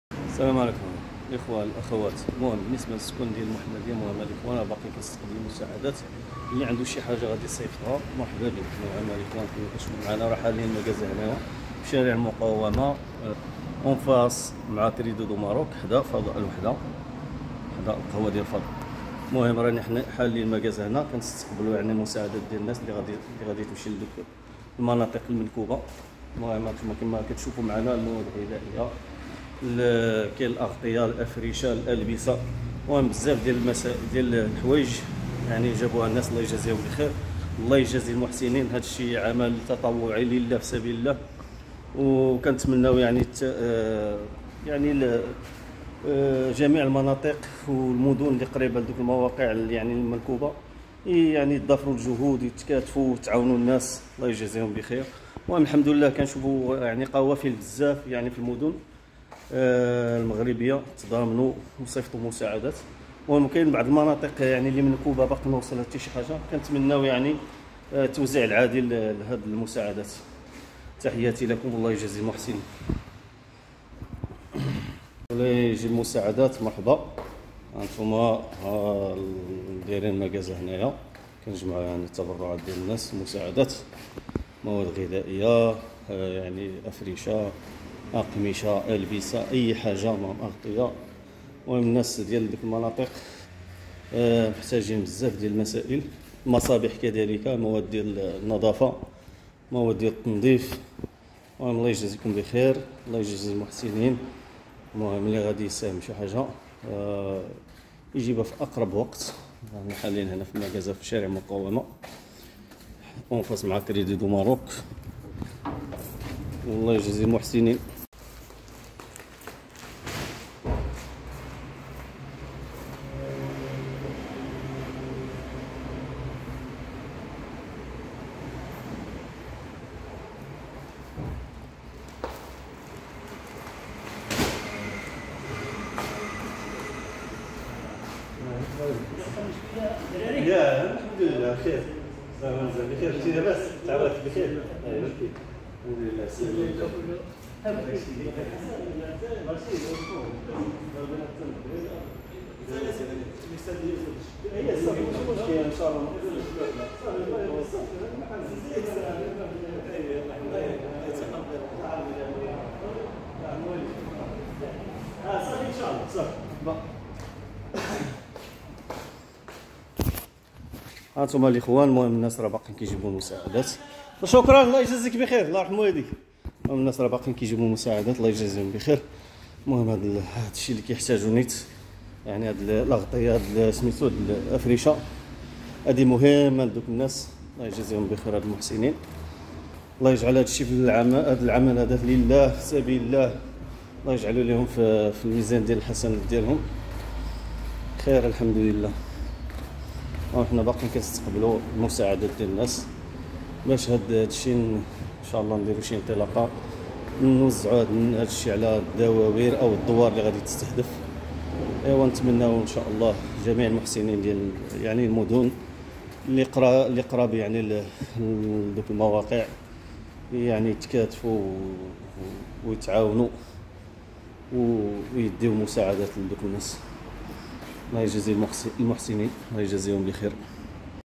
الحمدلله مازال كنتوصلوا بالمساعدات باش نستهدفوا الدواوئر المنسية
الحمدلله مازال كنتوصلوا بالمساعدات باش نستهدفوا الدواوئر المنسية واللب باقي ما وصلتهاش المساعدات